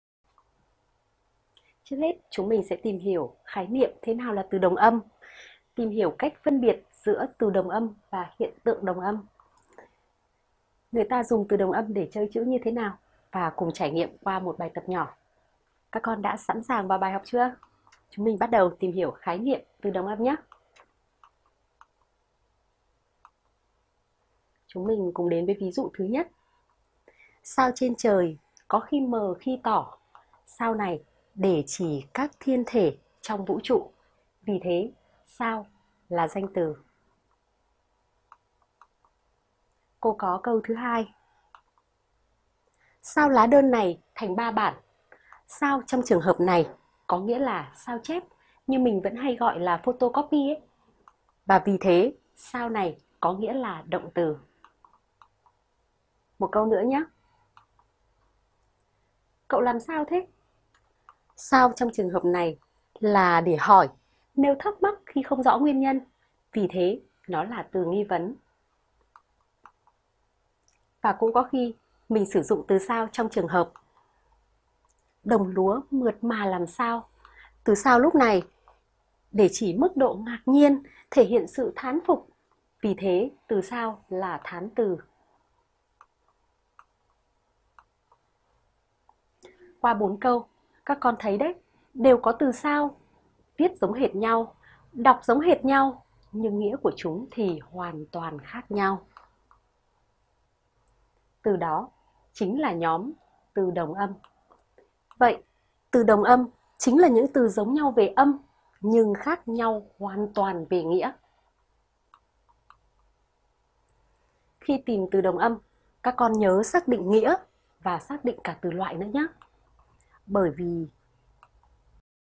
Từ đồng âm - Luyện từ và câu - Tiếng việt 5
Thế nào là từ đồng âm?
Ví dụ 1:
Sao trên trời có khi mờ khi tỏ.
Sao là Danh từ ⇒ Các thiên thể trong vũ trụ.
Ví dụ 2:
Sao lá đơn này thành ba bản.
Sao là Động từ ⇒ Tạo ra bản khác giống bản chính.
Ví dụ 2:
Sao cậu làm thế?
Sao là Từ nghi vấn ⇒ Nêu thắc mắc, không rõ nguyên nhân.
Học trực tuyến bài
TỪ ĐỒNG ÂM